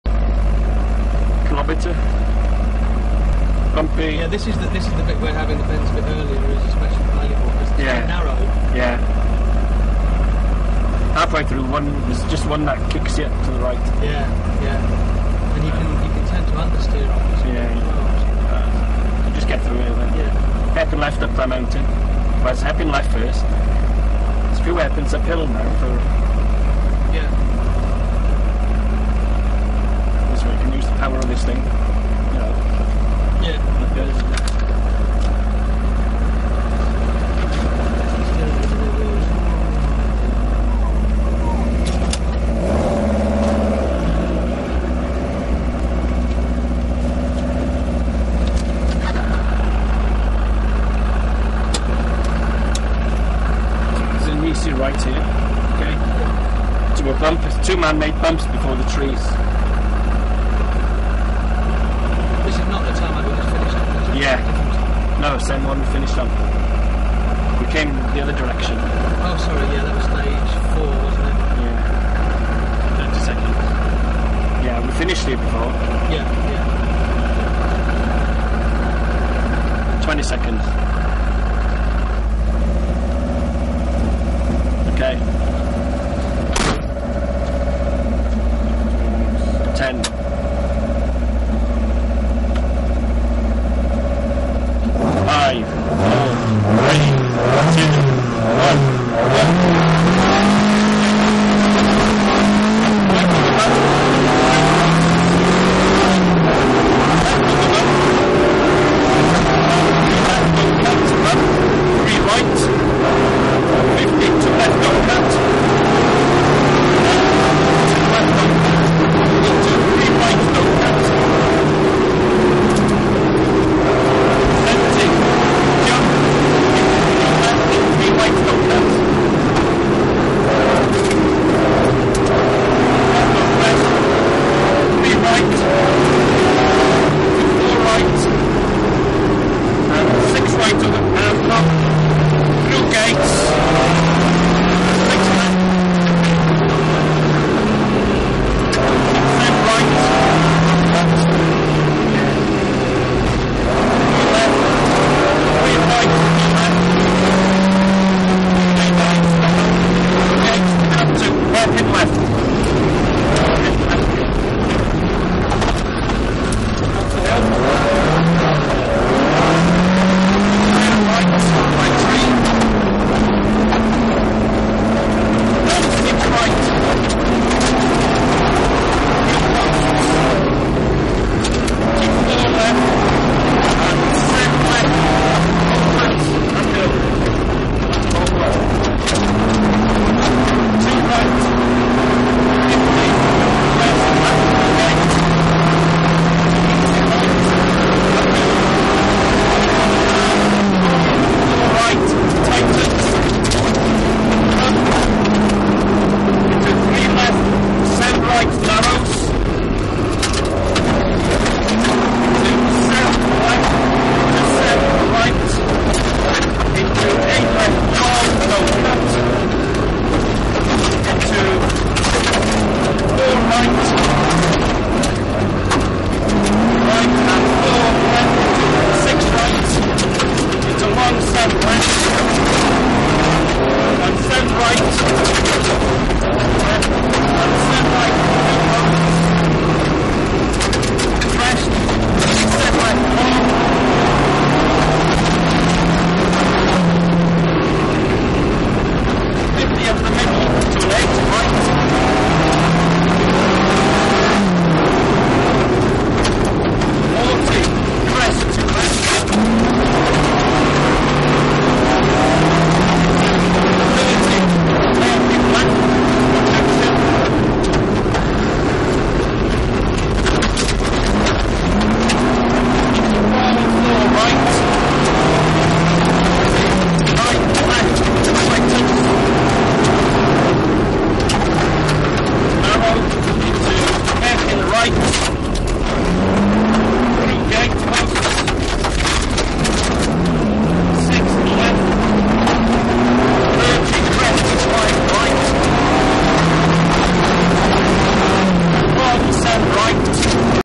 DVD_VIDEO_RECORDER 11 lip sync ok.avi
In car action - Dan Hart with Keith Davies from Bill Gwynne Coracle Stages at Sweet Lamb